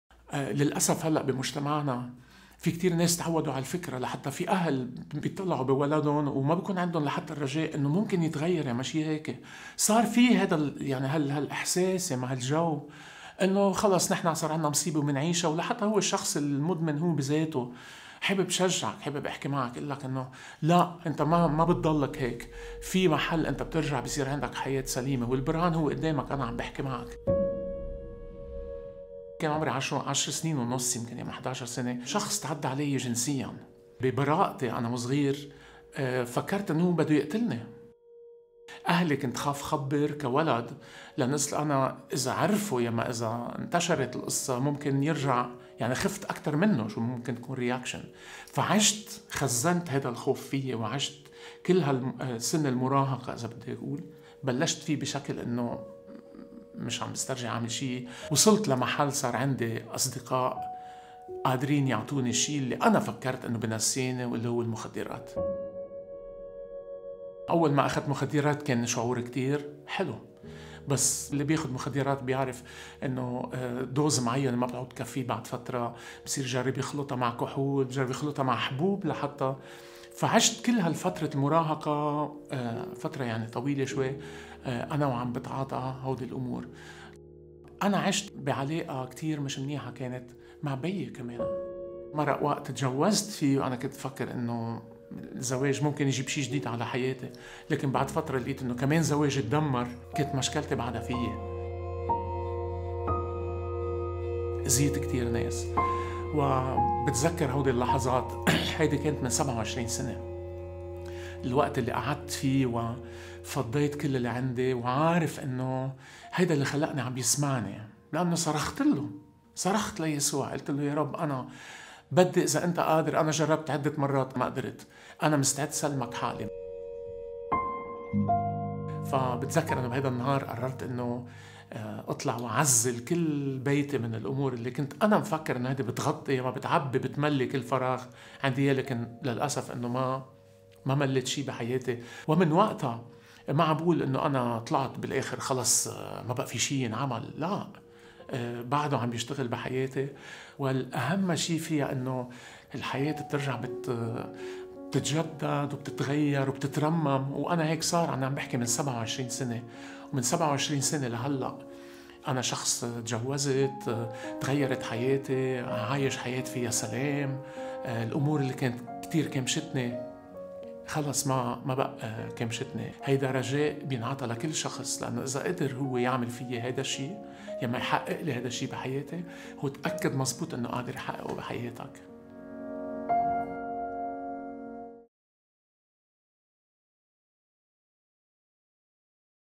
A story from the heart!
We all have a story to tell! A story that we keep most of the time for ourselves. Michel chose to share with the world and tell his own story to inspire others to change, to tell them that they deserve another chance.
Don't miss out watching the full video, share his own words, his message of hope and solidarity. Today it is your turn to prove to the world that everyone can fall but rise again!